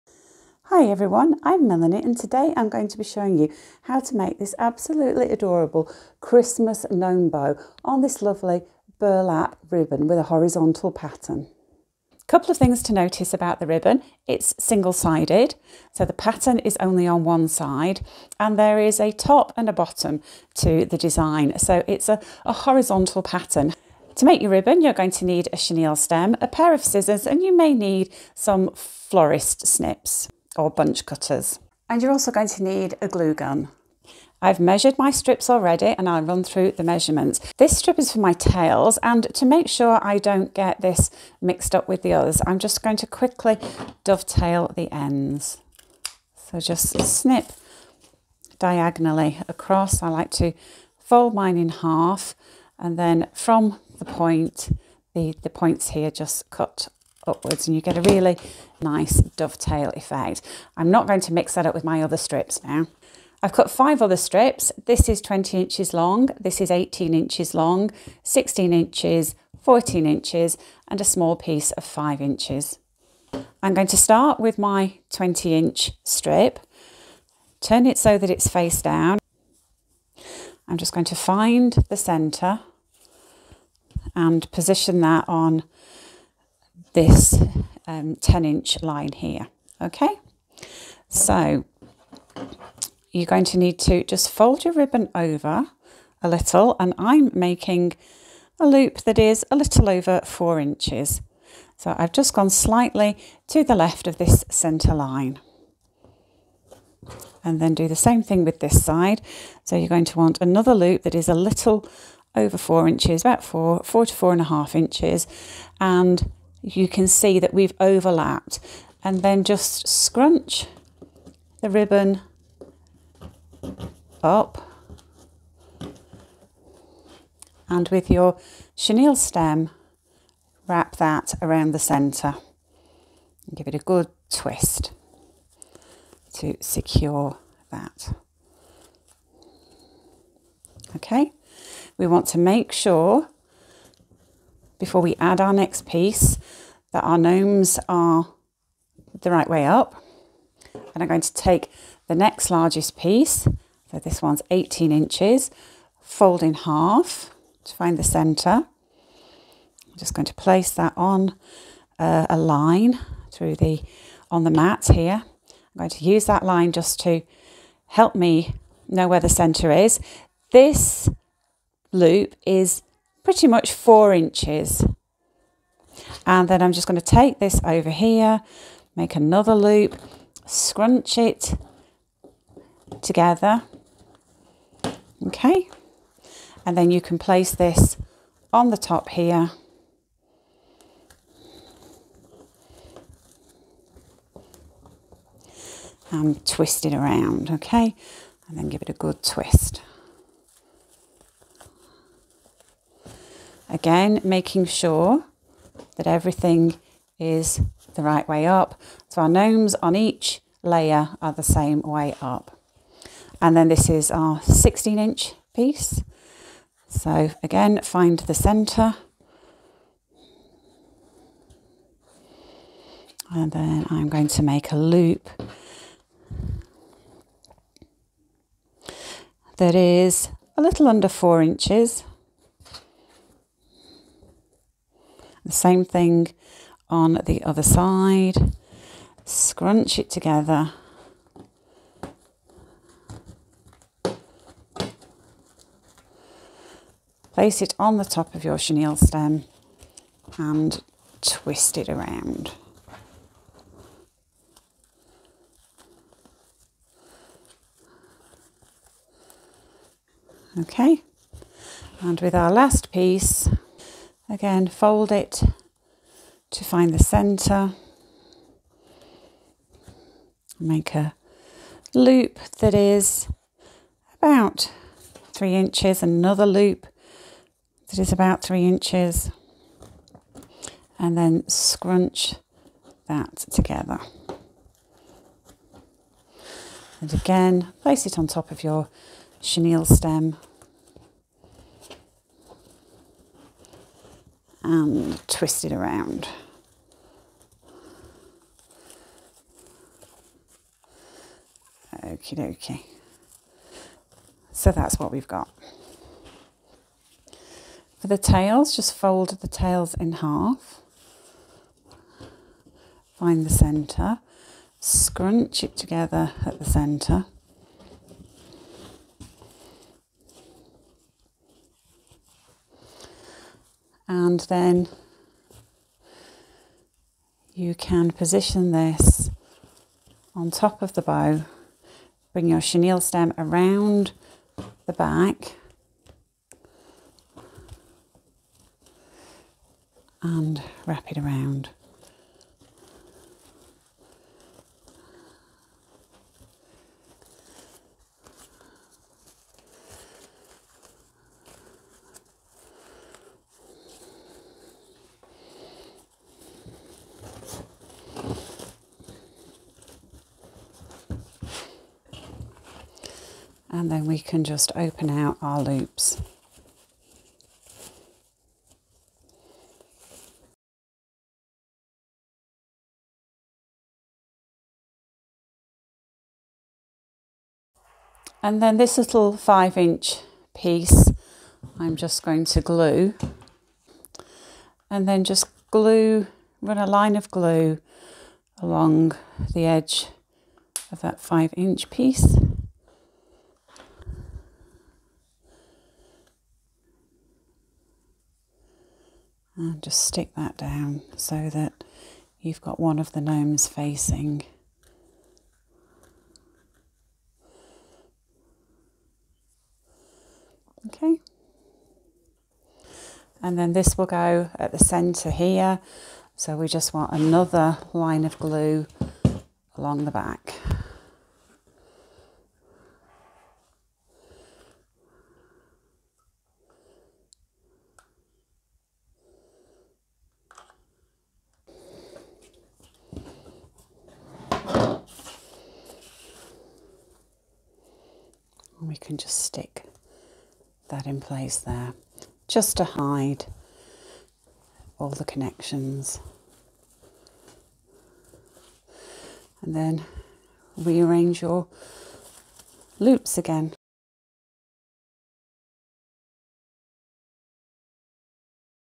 Make a Bow with One Sided, ONE DIRECTION Printed Ribbon 🎀 Make a Bow with One Sided Wired Ribbon
Learn how to make a bow with one sided, one direction printed ribbon. Making bows from ribbon with a horizontal pattern can be quite tricky - I show you a quick and easy technique.

I used 1.5 inch wide wire-edged single-sided ribbon with an adorable gnome pattern.

🎀 Oasis Stainless Steel Florist Scissors
🎀 Oasis Floral Snips / Bunch Cutters
🎀 Chenille Stems - multi-coloured
🎀 Chenille Stems - light brown
🎀 Chenille Stems - white
🎀 Ruler
🎀 Glue

*More Bow and Wreath Tutorials*